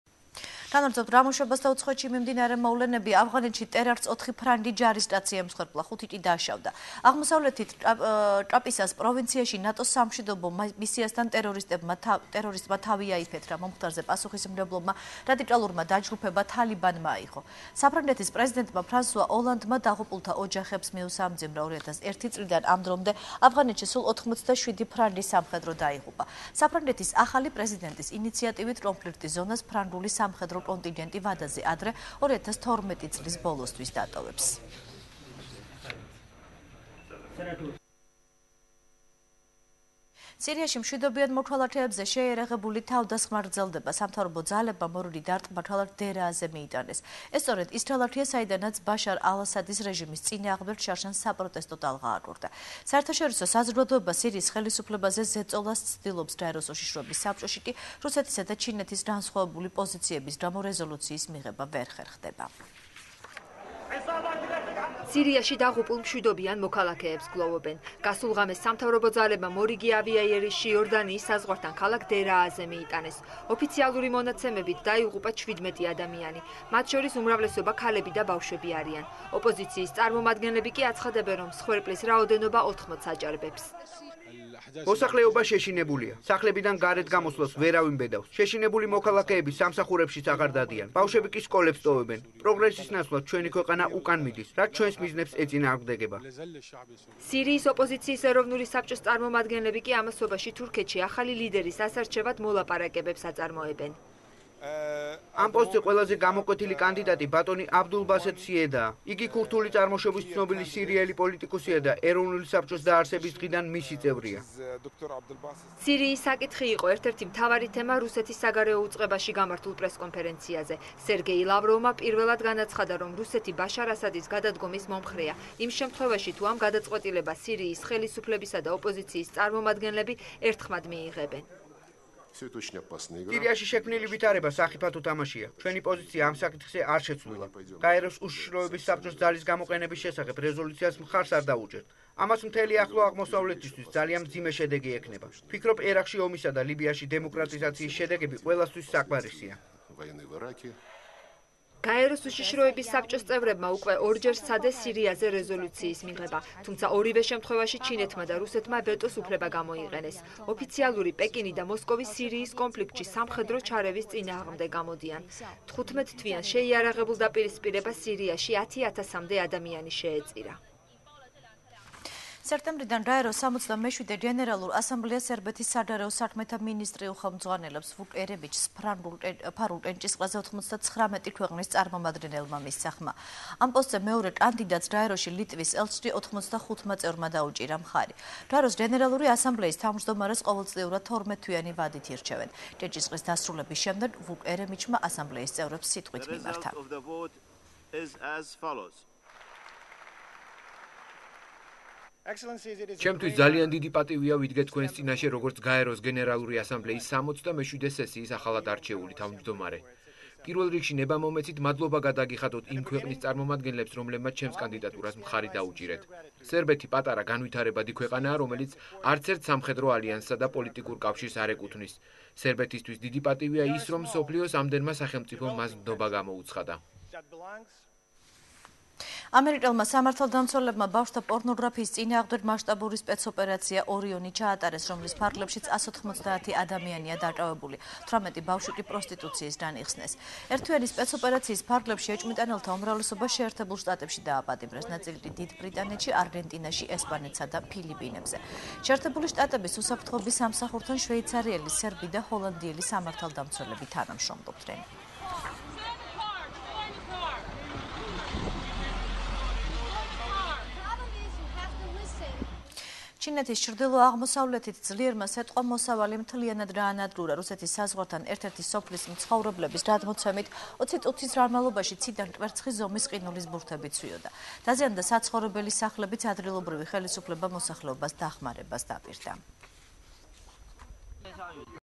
ახალი 9 | უცხოეთის ამბები | 09.06.12
ავღანეთში ტერაქტს ოთხი ფრანგი ჯარისკაცი ემსხვერპლა, ხუთი კი დაშავდა.
სირიაში მშვიდობიან მოქალაქეებზე შეიარაღებული თავდასხმა გრძელდება.
სექტემბრიდან გაეროს 67-ე გენერალურ ასამბლეას სერბეთის საგარეო საქმეთა მინისტრი უხელმძღვანელებს.
ამერიკელმა სამართალდამცველებმა ბავშვთა პორნოგრაფიის წინააღმდეგ მასშტაბური სპეცოპერაცია ,,ორიონი" ჩაატარეს, რომლის ფარგლებშიც 190 ადამიანი დააკავეს, 18 ბავშვი კი პროსტიტუციისგან იხსნეს.